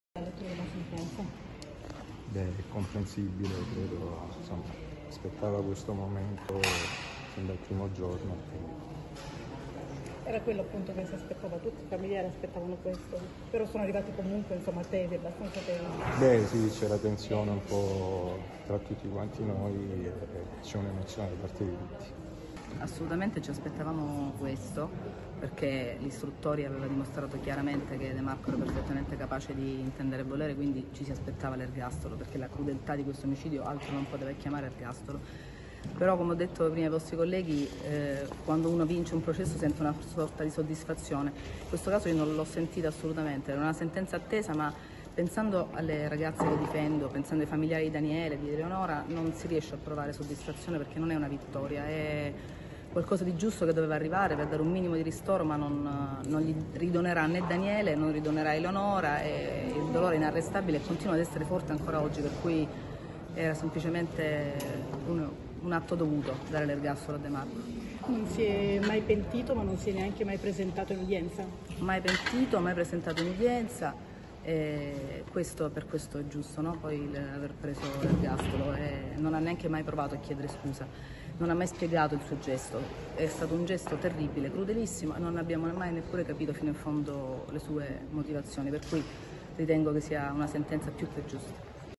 Via Montello, "De Marco? Capace di intendere e volere"
I legali di parte civile, soddisfatti per il verdetto e per la conclusione del primo grado del processo: «È una sentenza giusta, che non restituirà mai Eleonora e Daniele ai propri cari, ma che ci aspettavamo» è stato il commento unanime delle difese. «Ci aspettavamo questo - ha specificato l'avvocato Renata Minafra, che assiste le sorelle di Daniele - perché l'istruttoria ha dimostrato che De Marco è capace di intendere e volere. La crudeltà di questo omicidio non poteva essere valutata diversamente se non con l'ergastolo».   «La madre aspettava questo momento sin dal primo giorno», ha invece detto Francesco Spagnolo, avvocato della madre di Eleonora.